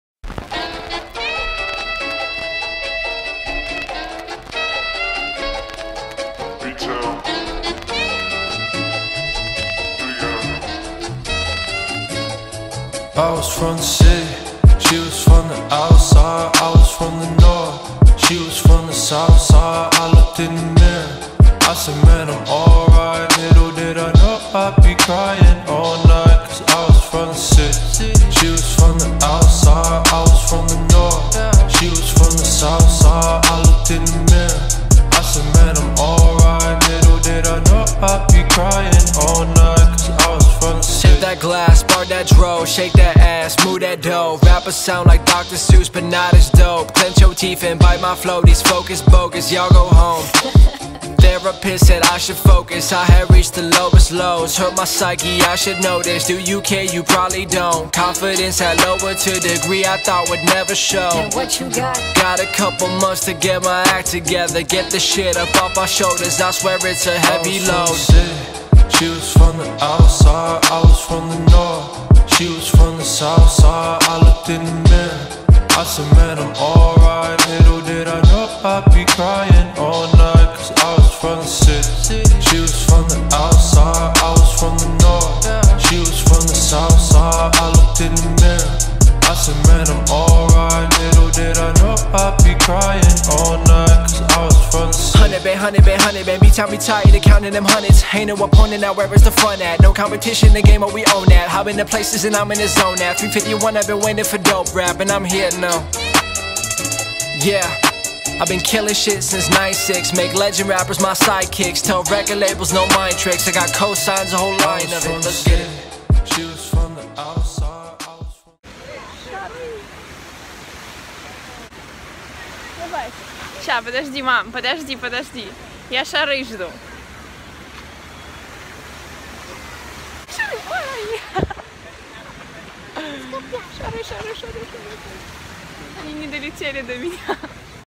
Spain X Barcelona
The second trip to Spain, to wonderful Barcelona city. Hope you like the vid.

If you like my videos go & check my pictures on Instagram: @ArinaRii

See you.  Xx